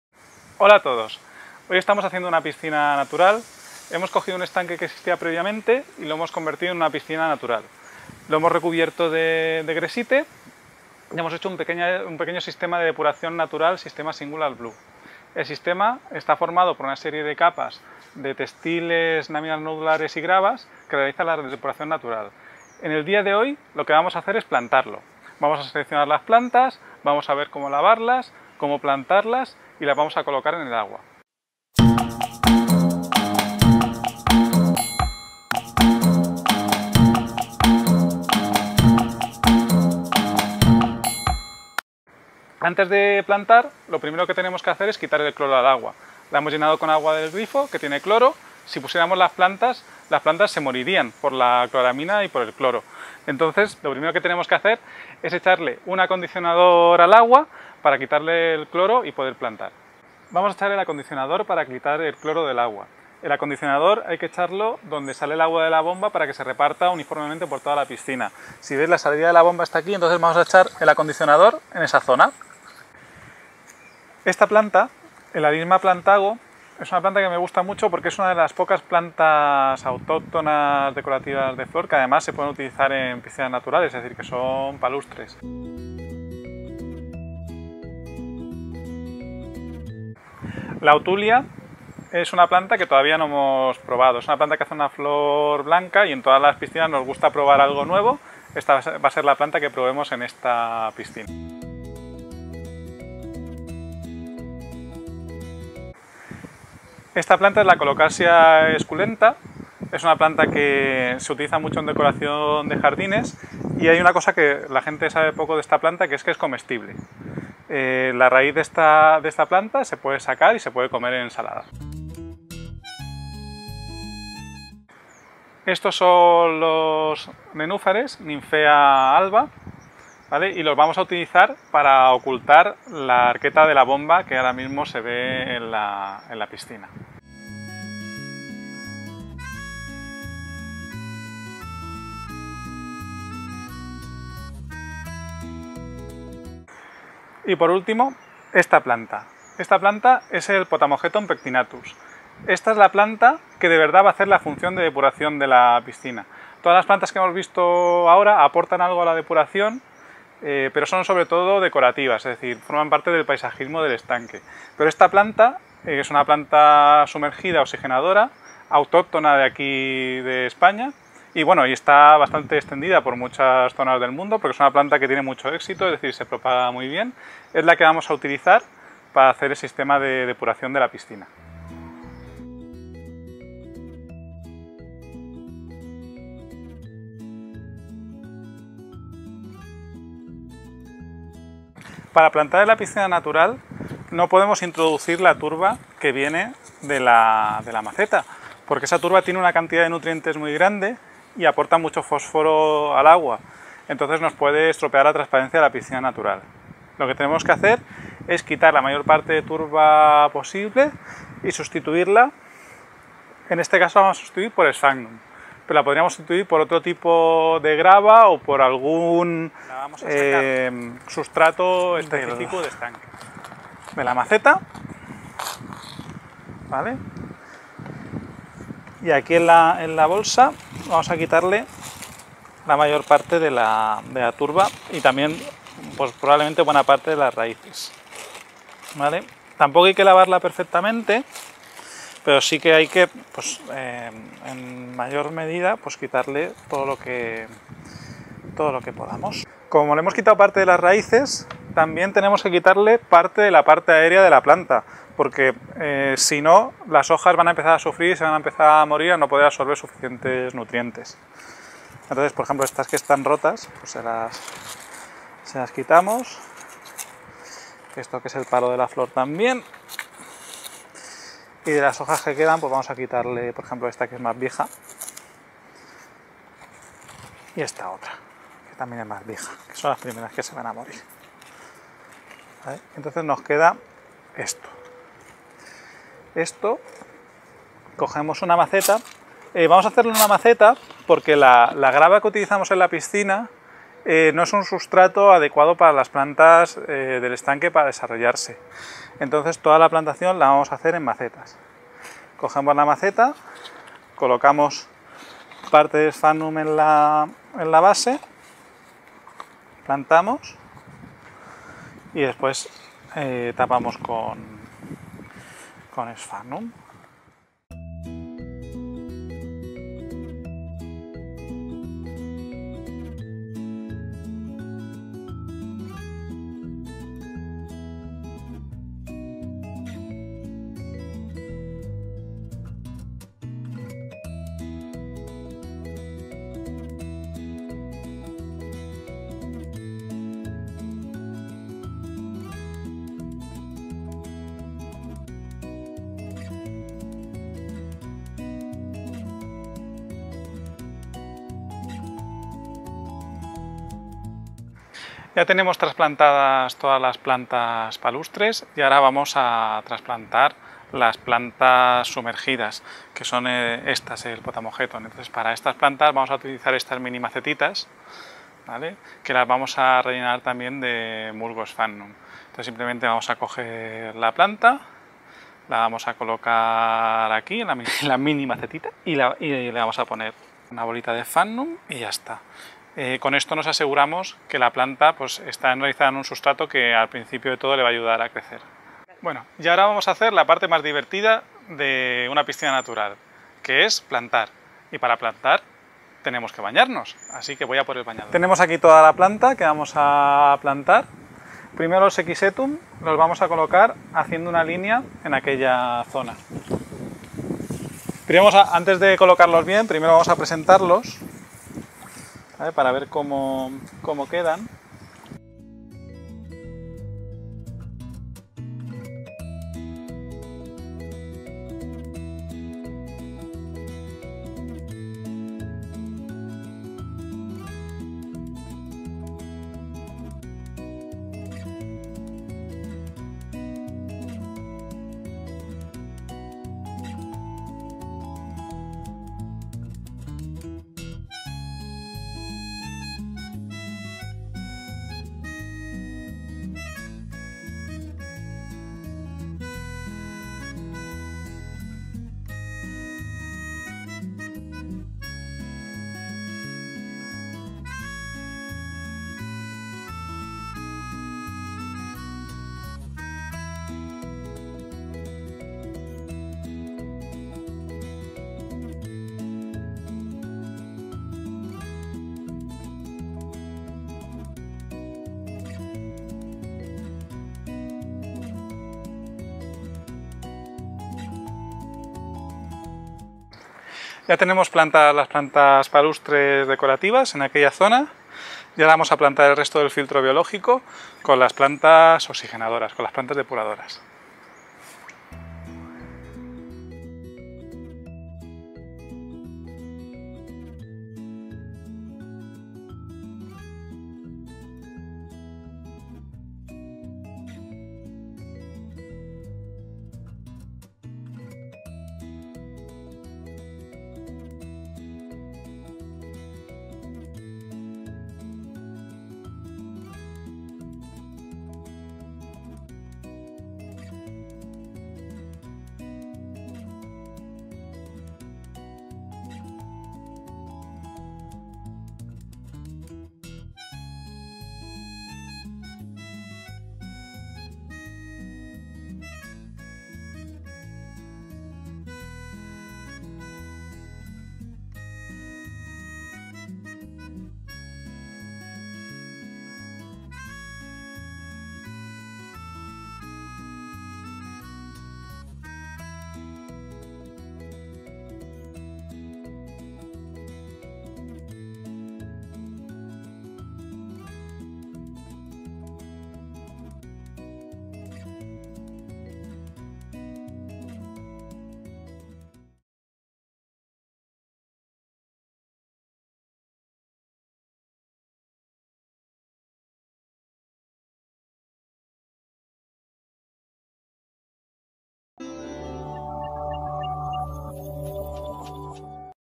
Plantar una biopiscina
¡Planta tu propia piscina natural! En el video de la semana pasada hablamos de la replantación de una piscina natural y dimos pequeñas pinceladas de como se realizaba esa replantación.  En este video os queremos contar con todo lujo de destalles como se realiza esa plantación para que no quede ningún tipo de duda.

👀Que veréis 👀
 🔵 0:48  Como quitarle el cloro al agua
 🔵 1:15 Selección de plantas
 🔵 3:14 Trabajo previo antes del plantar
 🔵 6:31 Plantación

 🎓 Formación online 🎓
Disponemos de formación online en jardines verticales, cubiertas vegetales y piscinas naturales aquí te dejamos los enlaces.